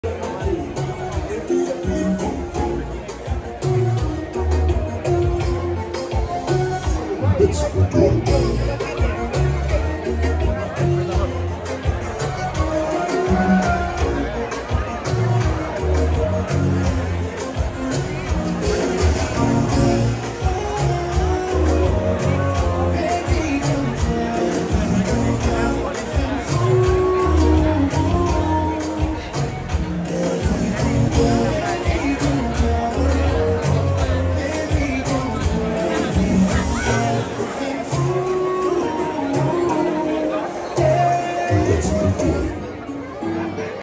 Kizomba Social & Oiseaux sur le balcon à la Soirée de danse à Crèmes Boboule le 28 juin 2024
Montreal, Canada